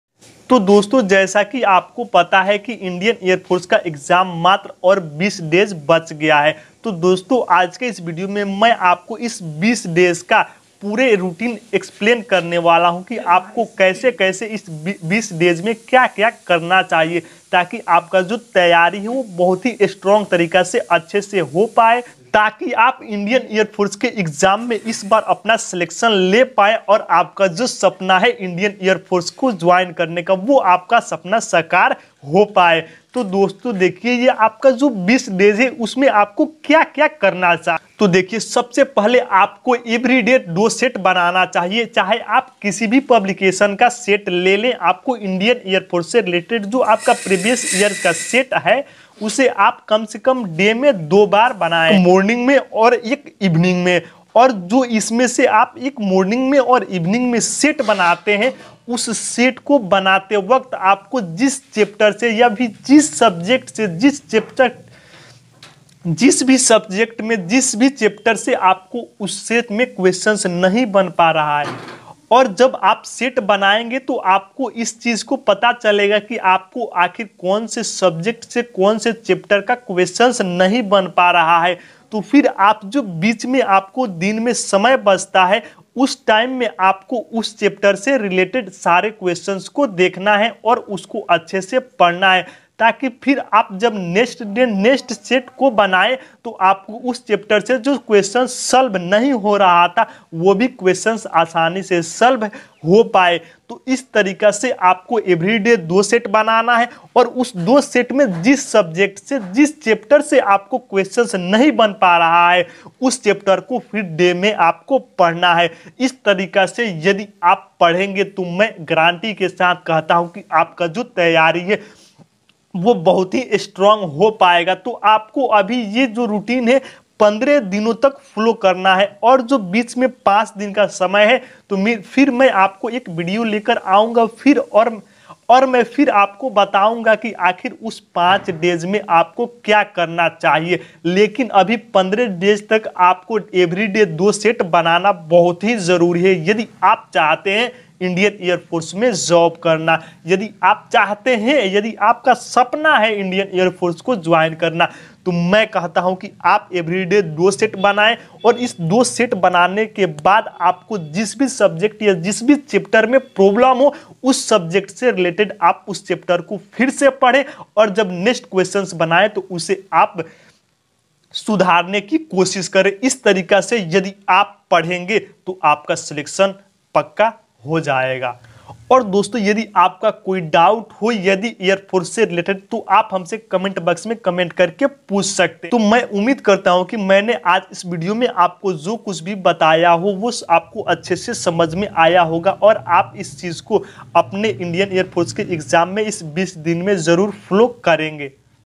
airforce exam date 2023 || airforce exam 20 days full strategy
Hello dosto 🤝
l hope you all are fine ☺️

dosto aaj ke esh video me hamne aap ko yah bataya hai ki airforce exam kewal 20 dine bacha hai ab esh 20 dine me kya padhna chahiye